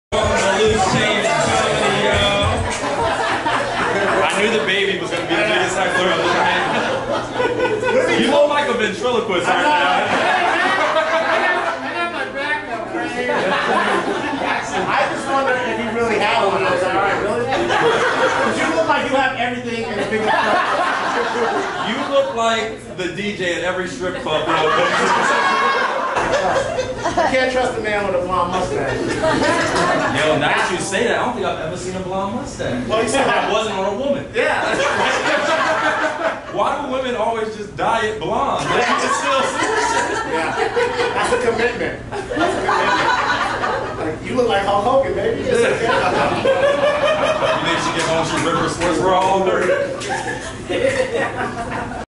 A Loose Change Comedy Cold Open
Gary Curtis and Mateen Stewart co-host and cold open Loose Change Comedy at its original location in Silverlake.  This is a cold open.  Now the show is held First Fridays at Vardagen in Venice.  Follow @LooseChangeComedy for updates and announcements!